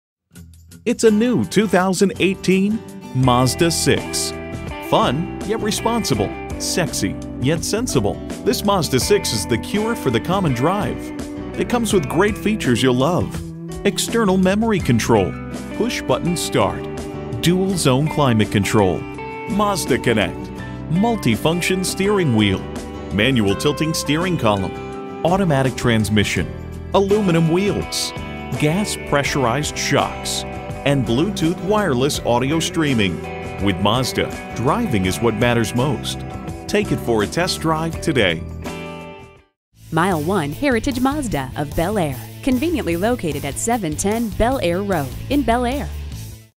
2018 Mazda Mazda6 Baltimore MD Bel-Air, MD F8326863 - SOLD
Year: 2018
Make: Mazda
Model: Mazda6
Trim: Sport
Engine: 2.5L 4-Cylinder
Transmission: Automatic
Color: Blue
Interior: Black
Stock #: F8326863
VIN: JM1GL1UM1J1326863

Address:
710 Bel Air Road